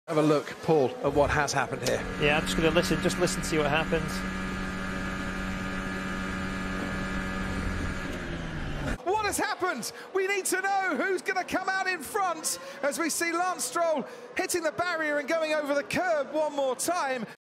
Lance Stroll Interrupts Max Verstappen Crash
A wild lance interrupts Max’s championship race